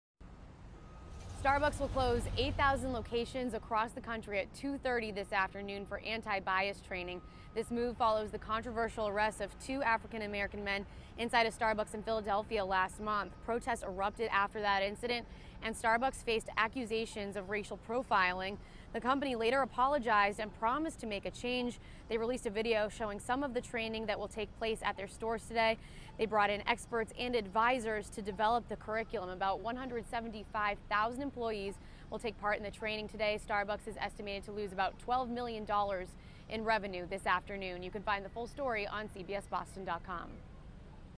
Reporter Update: Starbucks Closing For Anti-Bias Training
Thousands of Starbucks will be closed Tuesday afternoon. WBZ"s Anna Meiler explains why.